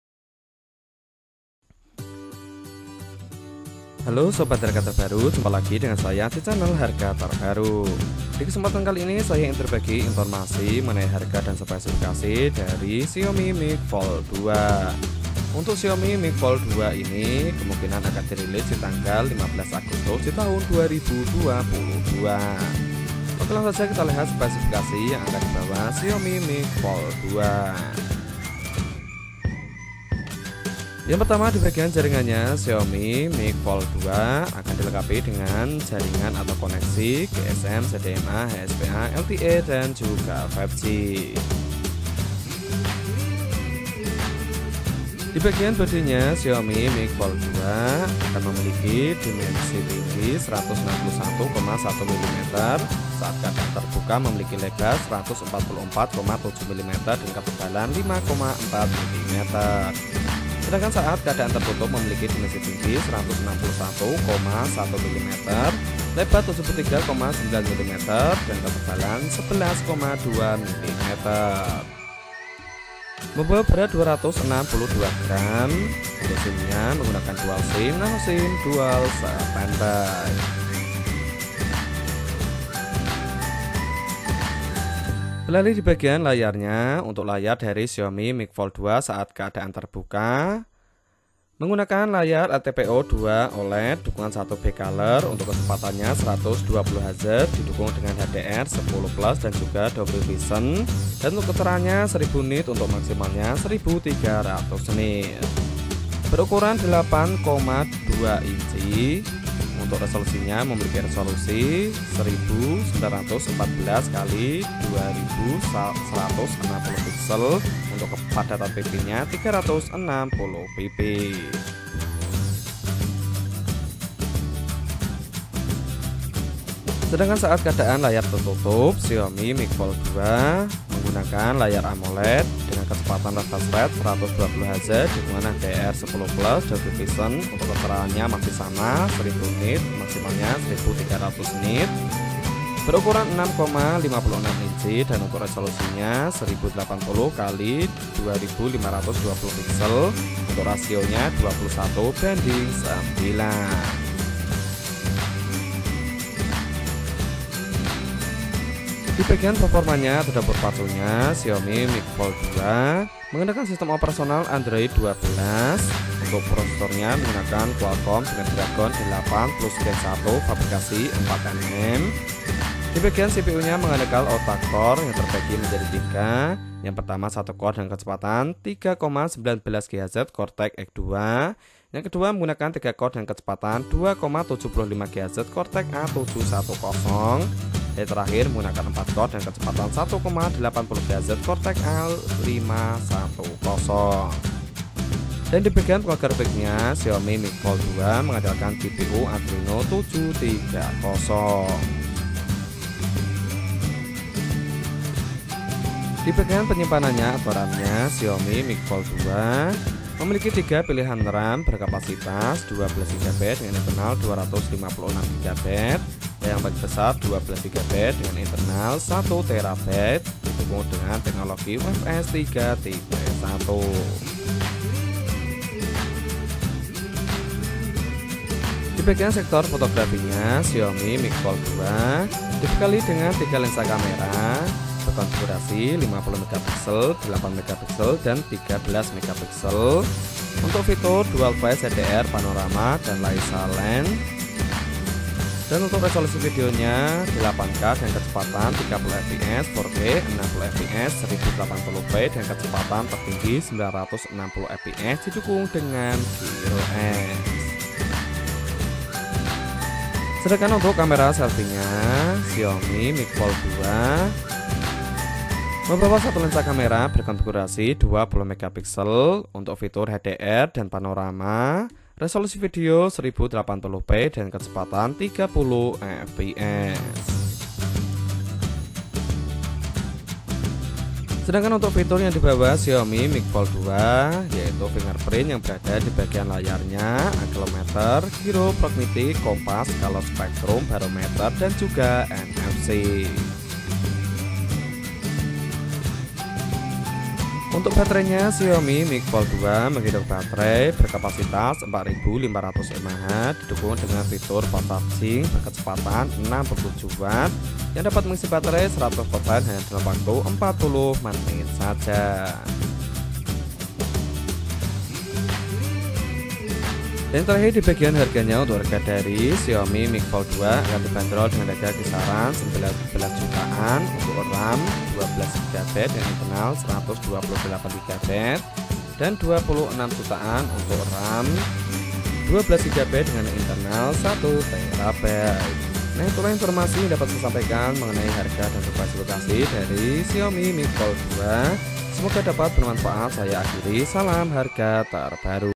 HARGA HP XIAOMI MIX FOLD 2 SPESIFIKASI LENGKAP
Xiaomi Mix Fold 2 menampilkan layar luar berjenis Amoled E5 berukuran 6,56 inci, refresh rate 120Hz, Dolby Vision, rasio aspek 21:9, kecerahan puncak 1.000 nits, dan dilapisi Corning Gorilla Glass Victus.

Sedangkan untuk layar utamanya berjenis LTPO 2.0 berukuran 8,2 inci, refresh rate 120Hz, gamut warna DCI-P3, resolusi 2K+ atau 2.160x1.914 piksel, dan kecerahan puncak 1.300 nits.

Xiaomi Mix Fold 2 dipacu prosesor Snapdragon 8+ Gen 1 dengan opsi penyimpanan 12GB/256GB, 12GB/512GB, dan 12GB/1TB. Ponsel lipat ini menjalankan sistem operasi MIUI Fold 13 berbasis Android 12.